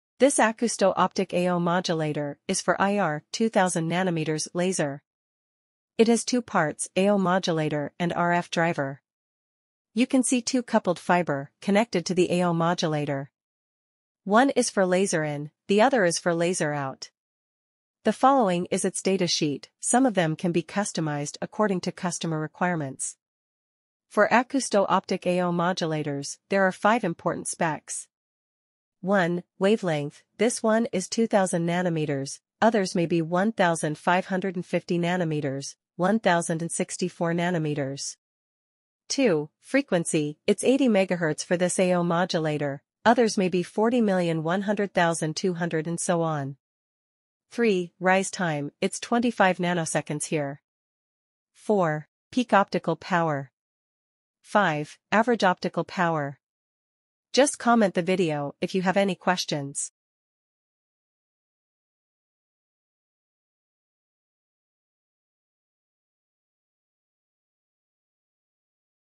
Acousto optic Modulator IR AOM Fiber Coupled 2000nm Peak Optical Power Pulse 10kW
It has two parts , AO Modulator and RF driver . You can see two coupled fiber connected to the AO modulator. 1 is for laser in, the other is for laser out .

The following is its datasheet, some of them can be customized according to customer requirements.

For Acousto optic AO Modulators, there are 5 important specs:

1 wavelength, this one is 2000nm, others may be 1550nm , 1064nm.
2 Frequency, it's 80MHz for this AO modulator, others may be 40Mhz,100Mhz,200Mhz and so on.
3 Rise Time, it's 2.5dB here
4 Peak Optical Power
5 Average Optical Power

Interaction Material:Tellurium dioxide
Wavelength:2000nm (IR AOM)
Operating Wavelength Range:+-30nm
Frequency:80MHz
Ultrasonic Velocity:4200m/s
Insertion Loss:2.5dB
Extinction Ratio(Turn On / Off):55dB
Polarization Extinction Ratio:18dB
Rise-Time/Fall-Time:25ns
Return Loss：40dB
Voltage Standing Wave Ratio (VSWR): 1.5:1
Average Optical Power:0.5W
Peak Optical Power (Pulse): 1kW
Input impedance:50
RF Power:2.5W
Device Interface:SMA Fiber
Fiber Type:PM1550
Fiber Length:1m
Fiber Connector:FC/APC